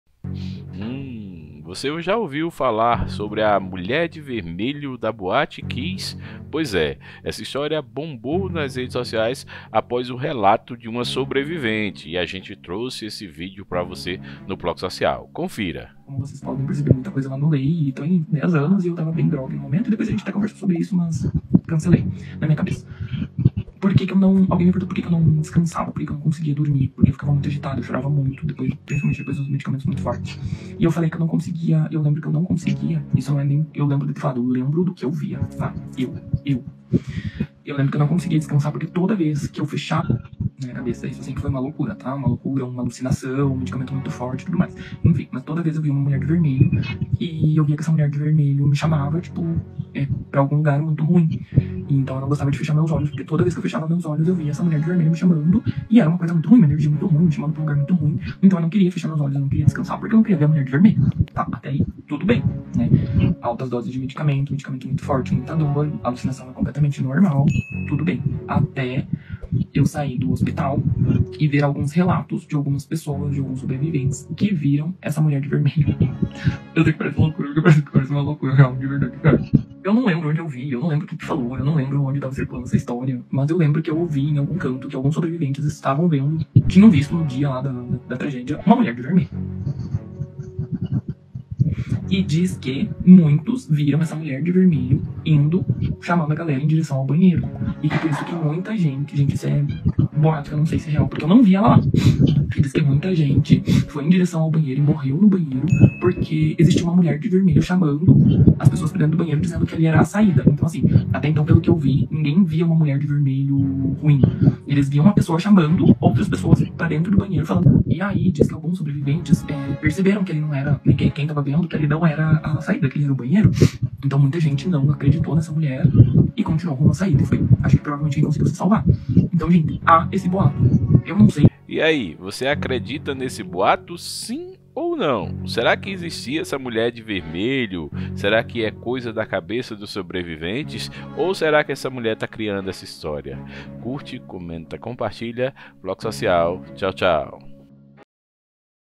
A história da Mulher de vermelho da Boate Kiss • Depoimento de sobrevivente verdade ou mentira
Depoimento de sobrevivente da Boate Kiss sobre uma suposta mulher de vermelho que ela viu durante tratamento e também em relatos de outros sobreviventes.

Verdade ou mentira? O que você acha?